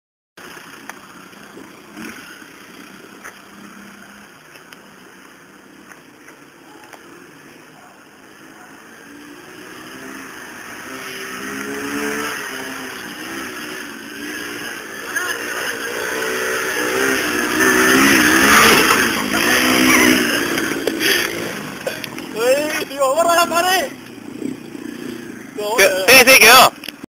Poli saltando en el TRX 90 honda cuatriciclo
Presten atencion a la caida.